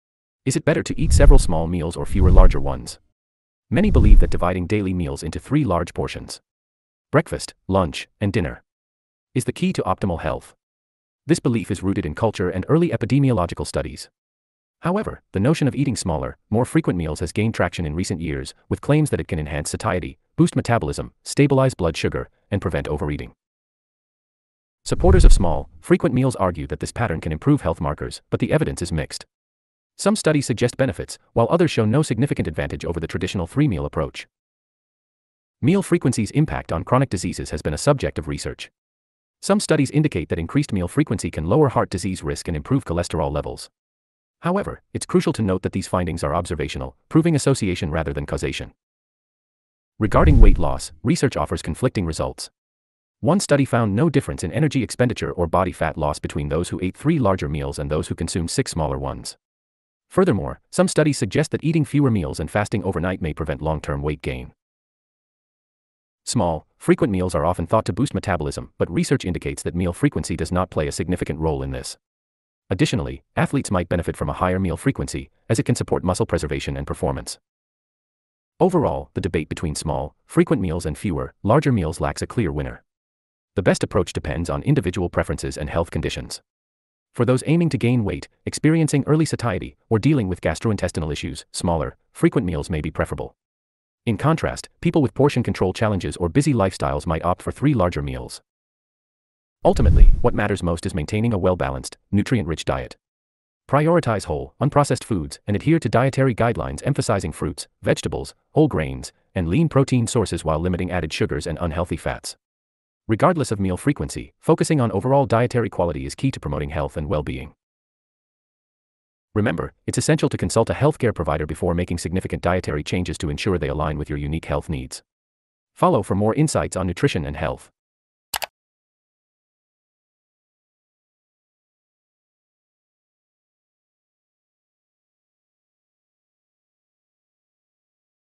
Is it better to eat several small meals or fewer larger ones?
Are you confused about whether it's better to eat several small meals or fewer larger ones? Watch this video to find out which eating pattern can help you achieve your health goals. We'll explore the benefits of both approaches and give you tips on how to make the most of your meals. Don't miss out on this informative video!

Try Outlive Health's premium health supplements at outlivehealth.us